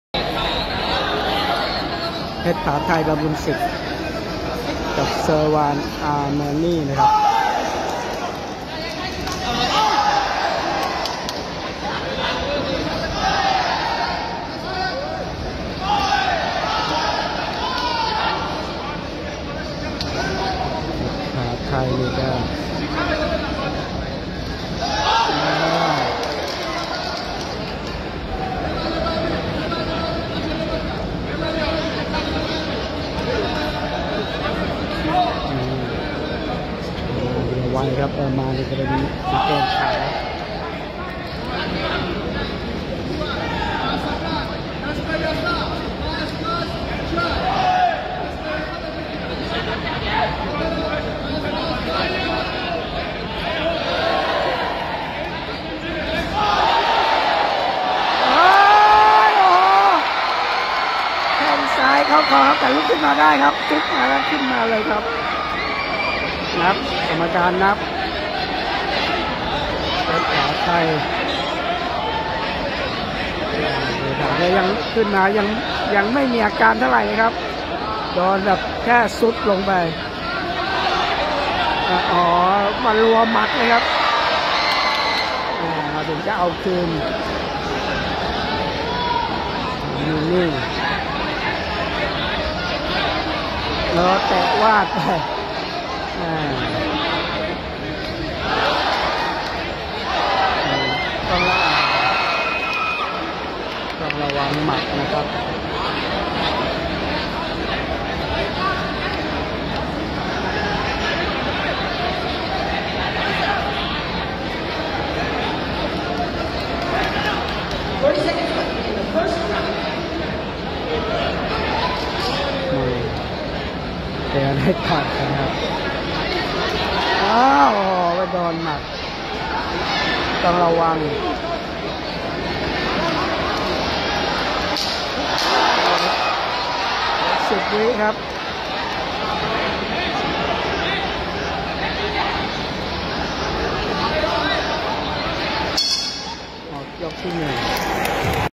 เพชรผาไทยเตะคอได้นับ (คลิปยก1)
เพชรผาไทย บํารุงศิษย์  เตะคอได้นับยก 1 แต่ยก 2 ถูกหมัดเซอร์วาน อามินี  นักชกอิหร่าน โดนนับคืน ตัดสินยก 3  ชนะคะแนน เซอร์วาน อามินี  นักชกอิหร่าน (คลิปยก1) LUMPINEE 70" เวทีมวยลุมพินี วันศุกร์ที่ 12  กรกฎาคม 2567

►กลุ่มเม้าท์มวย แลกปลี่ยนข่าวสาร แลกเปลี่ยนความคิดเรื่องมวย :